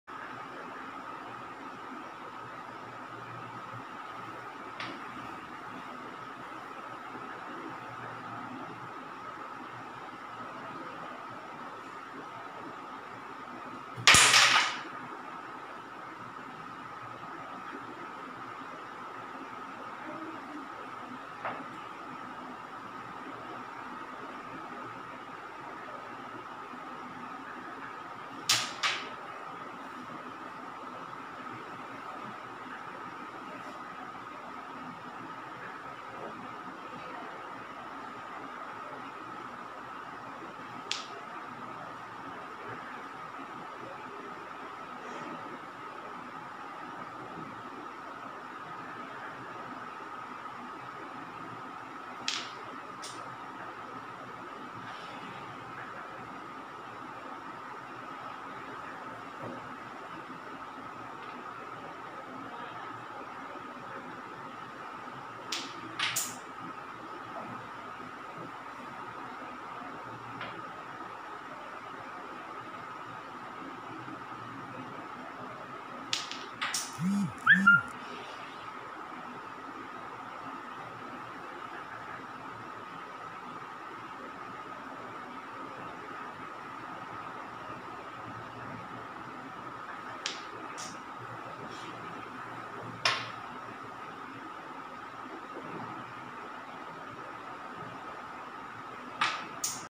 S79 | Slam No. 79 | Carrom White Slam by E. Magimairaj
E. Magimairaj is an International and National Carrom Champion

International Carrom Federation Cup London - 1997 Winner

His passion for the game never ends🎉🎉

Carrom Chennai - Corporate Carrom Coaching